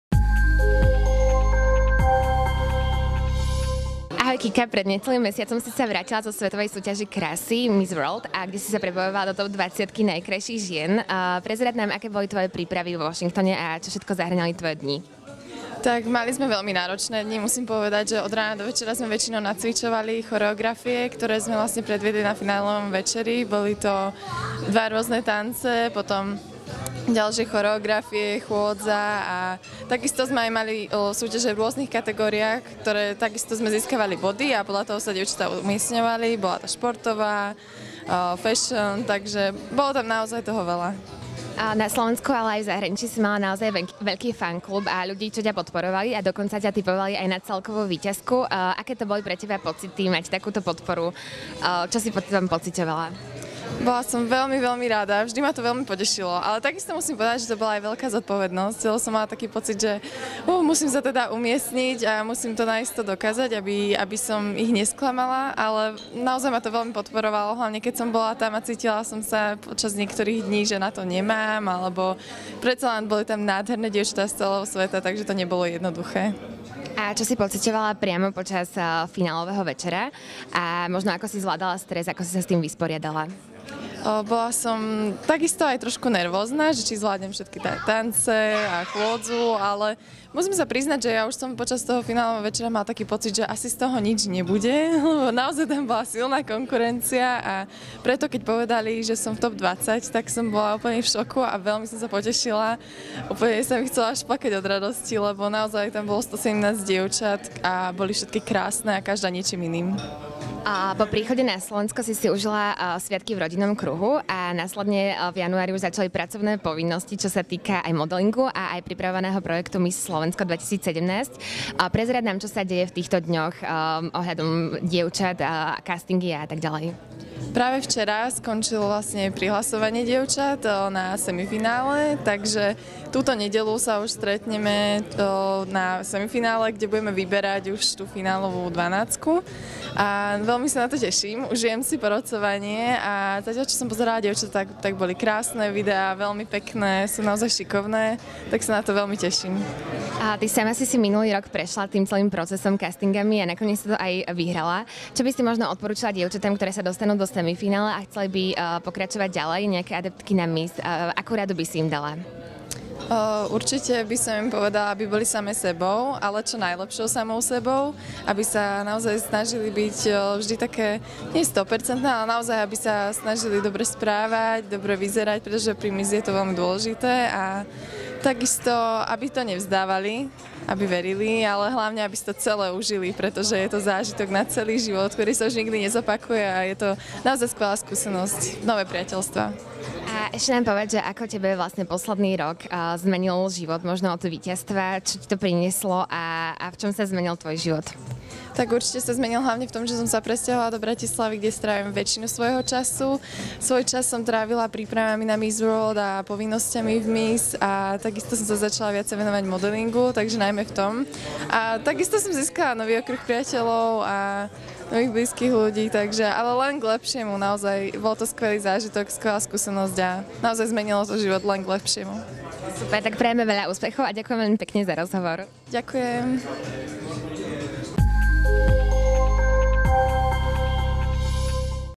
Najkrajšia Slovenka Kristína Činčurová sa prebojovala do TOP 20
Ako prebiehalo svetové finále?